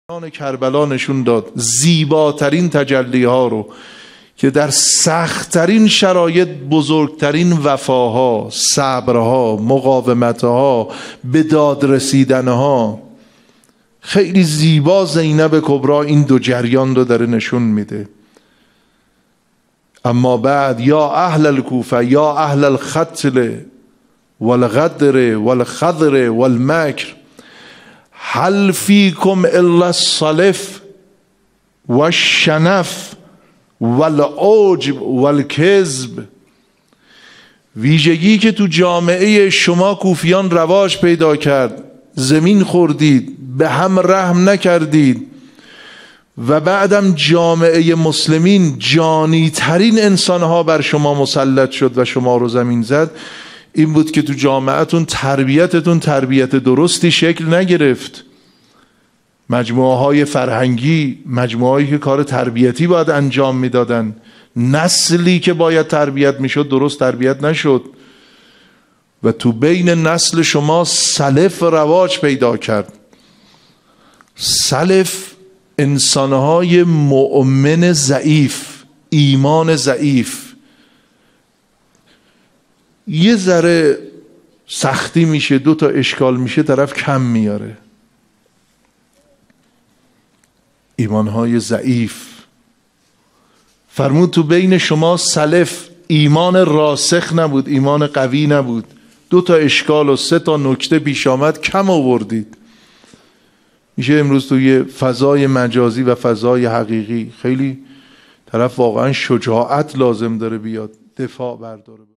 تربیت جامعه - حجت الاسلام میرهاشم حسینی
کربلا زیباترین تجلی ها

 در زیبا ترین صبر ها مقاوت ها به داد رسیدن ها

خیلی زیبا زینب کبری این دو جربان نشان می دهند

ویژگی جامعه کوفه جه بود؟

این بود که در جامعتون تربیتی درست شکل نگرفت

تکیه حسینیه‌ای به وسعت ایران